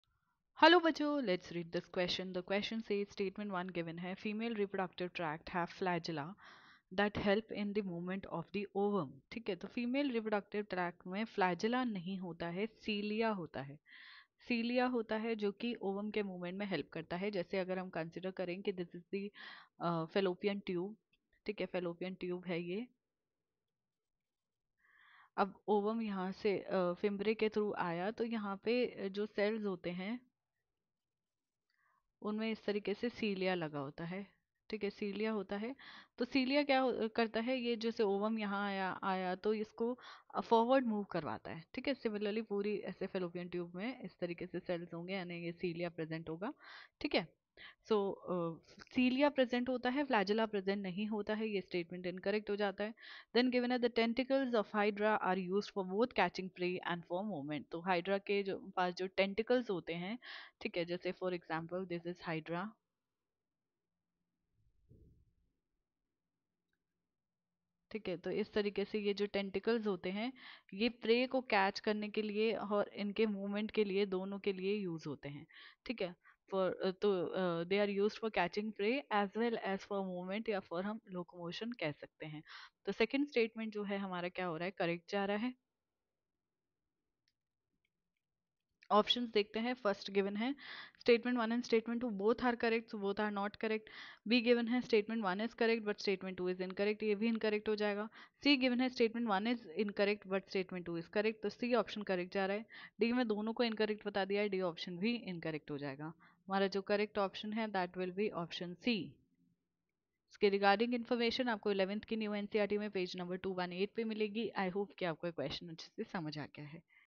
Statement-I: Female reproductive tracts have flagella that help in the movement of the ovum.\nSt....
Statement-I: Female reproductive tracts have flagella that help in the movement of the ovum.\nStatement-II: The tentacles of the Hydra are used for both catching prey and for movement.